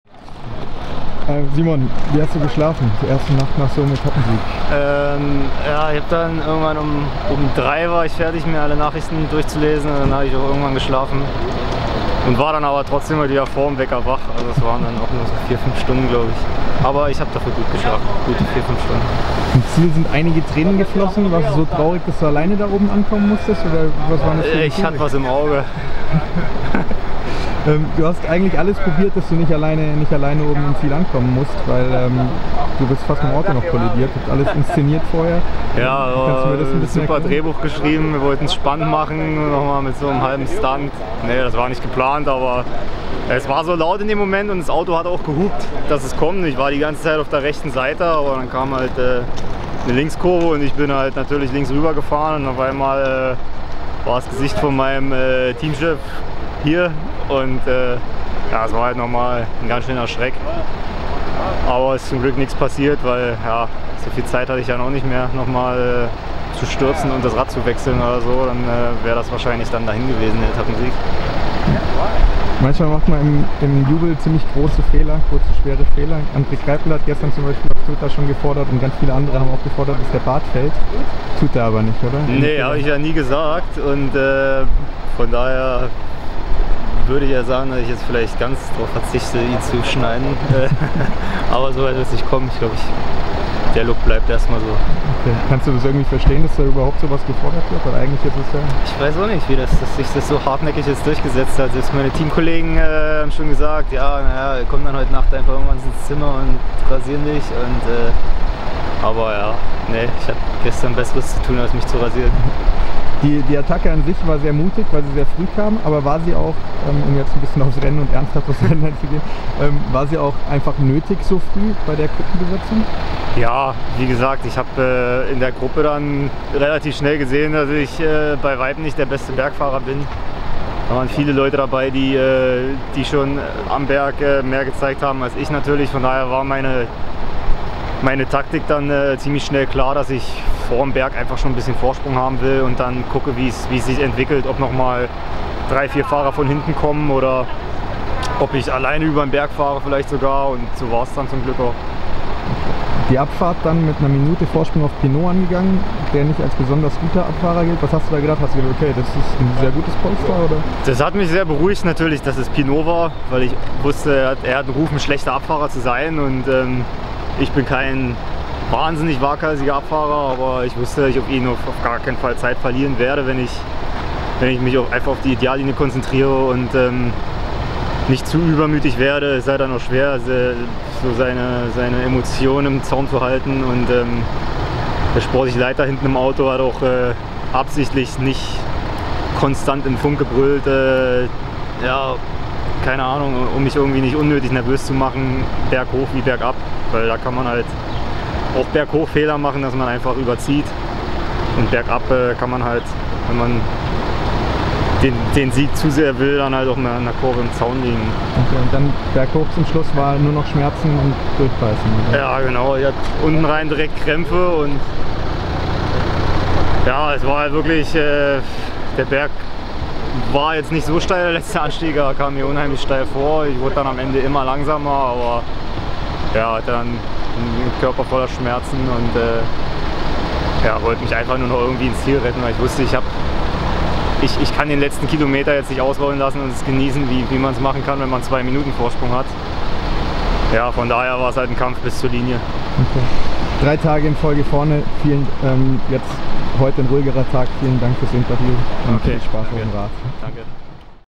Geschke nach emotionalem Sieg: "Ich hatte was im Auge"
Simon Geschke spricht am Morgen nach seinem sensationellen Etappensieg von Pra Loup vor dem Teamhotel von Giant-Alpecin über den großen Erfolg - teilweise nicht ganz ernst, dafür aber umso lockerer: "Wir haben ein super Drehbuch geschrieben und wollten es spannend machen mit so einem halben Stunt..."